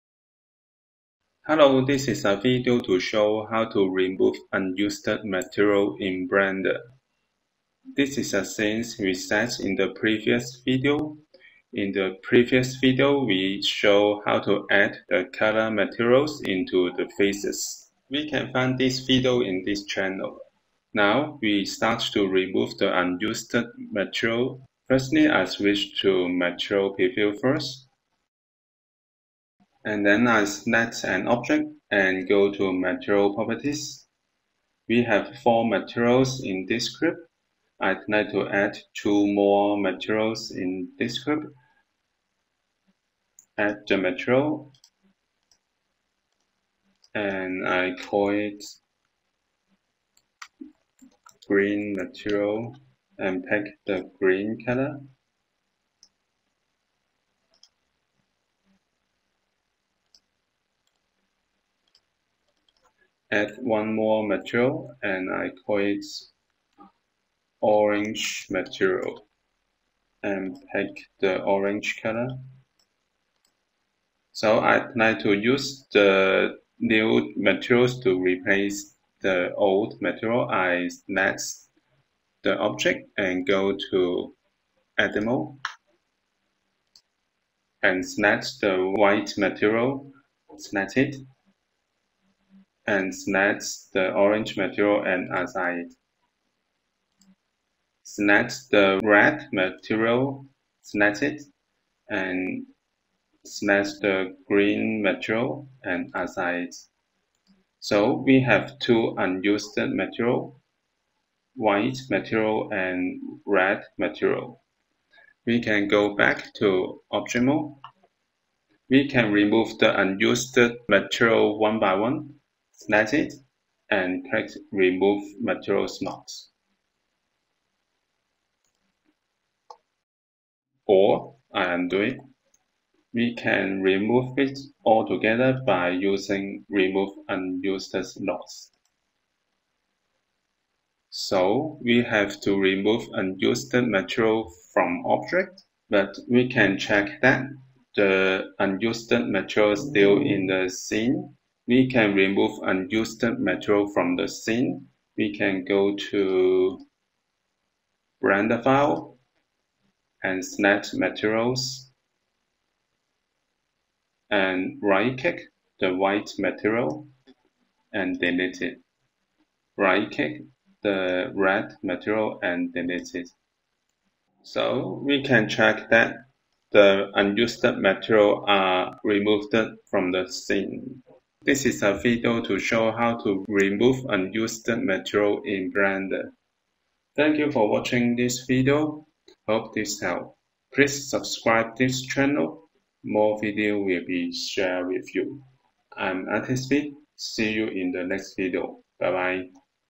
Blender how to remove materials from project
Blender tutorial to show how to remove unused materials from project, this is working for Blender 3.0 or later. Sometimes after adding materials and changing to material, we can delete the materials by using remove material slot in the list. In case, there are too many material in Blender scene, we also can remove all material from the scene. Hope this helps.

Timestamp

0:00 Intro –Blender remove unused material from project
0:07 Start from coloured 3D cube
0:34 Replace Materials
1:50 Remove Unused Material from Object
2:34 Remove Unused Material from Project

Let's create with Blender!